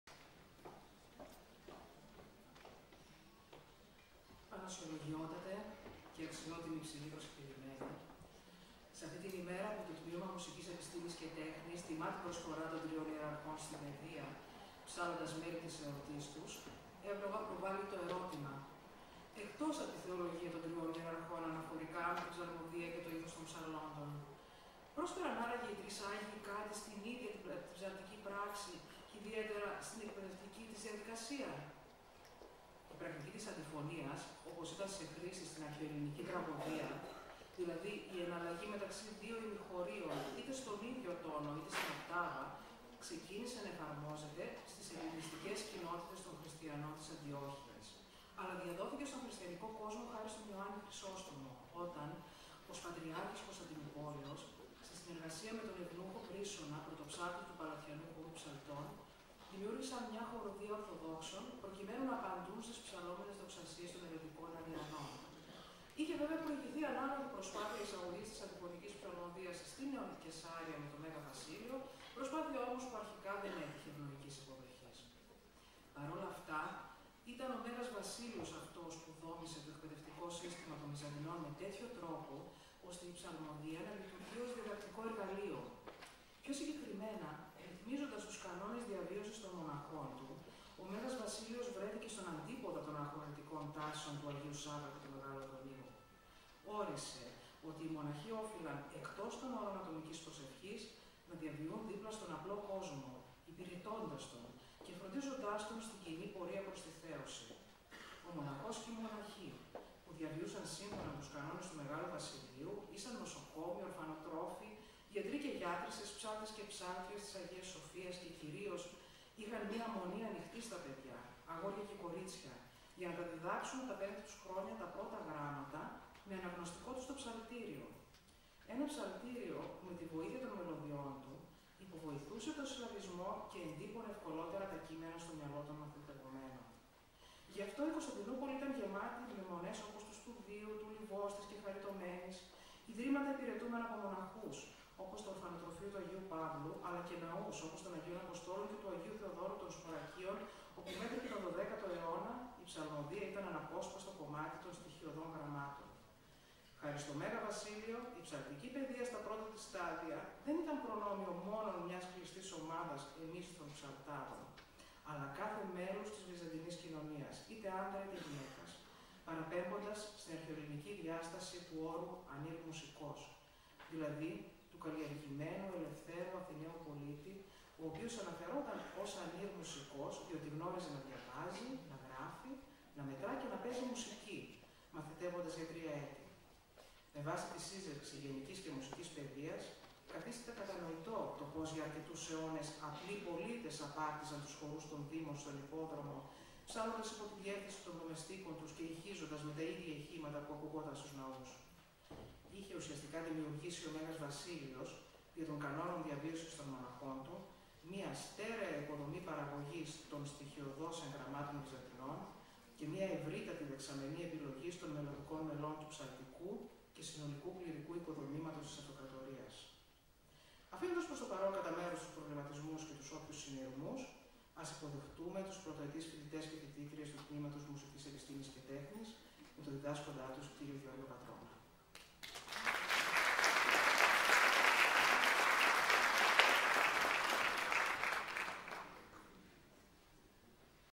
2 ΠΡΟΛΟΓΟΣ
Eισαγωγικά σχόλια από τη συνδιδάσκουσα Δρ. Ευαγγελία Σπυράκου (μέλος ΕΕΠ ΤΜΕΤ).